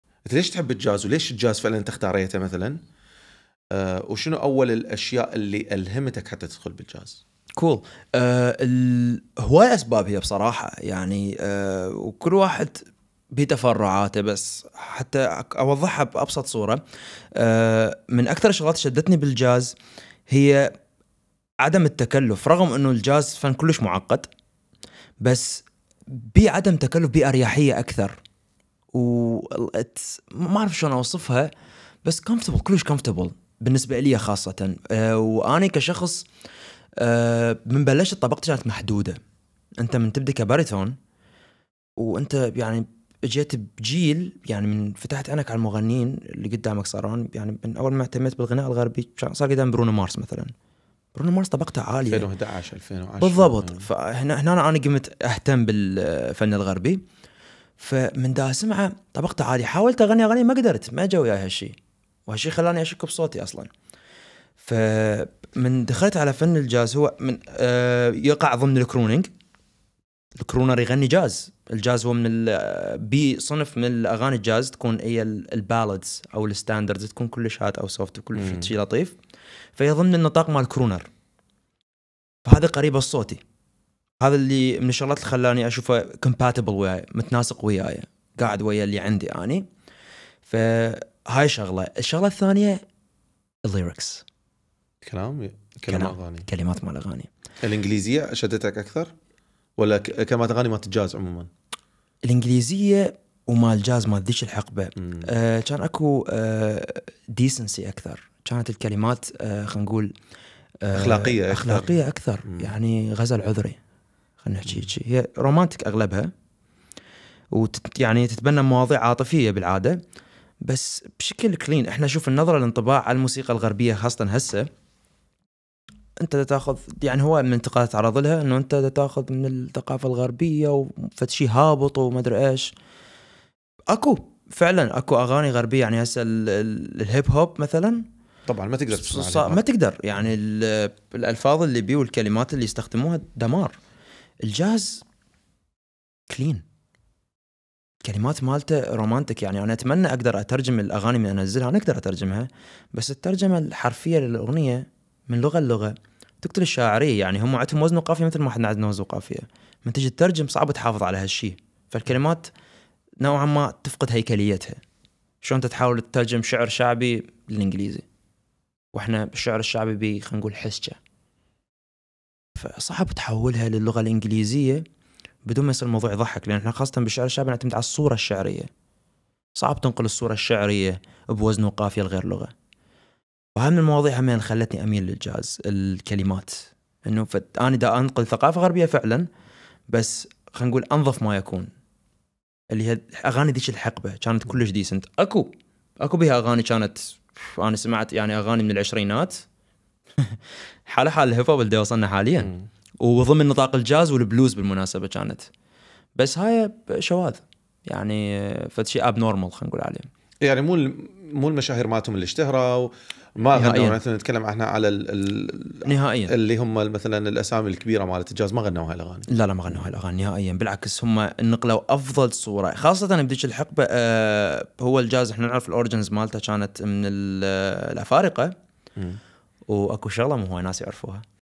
لماذا أخترت موسيقى الجاز؟ | بودكاست شنو القصة مع الفنان محمود طالب
الحلقة الكاملة

محمود طالب هو فنان عراقي شاب متخصص في غناء الجاز مميز جداّ بطبقات صوته الهادئة والتي كونت اسلوبه بالغناء. درس الموسيقى والغناء في كلية الفنون الجميلة في بغداد وطور أسلوبه بالغناء ليصل بنجاح الى فئات المجتمع المهتمة في هذا المجال.

تحدثنا في هذه الحلقة من بودكاست شنو القصة مع الفنان محمود طالب عن الموسيقى بشكل عام وموسيقى الجاز بشكل خاص ومراحل تطوير هذه الموسيقى الهجينة. كما تكلمنا عن بدايات محمود في فهمه للمدى الصوتي وتطوير صوته والالهامات الاولى في الموسيقى والجاز. تحدثنا ايضا عن الطبقات الصوتية، الفرق بين الموسيقى الغربية والشرقية وبالتأكيد حادثة برنامج Yooz Talent و"طلع ايدك من جيبك وأحمد البشير وحسام الرسام.

حساب بودكاست شنو القصة؟ على الانستغرام

يمكنكم ايضا الاستماع لبودكاست شنو القصة؟ على منصات أبل وجوجل وسوبوتيفاي وانغامي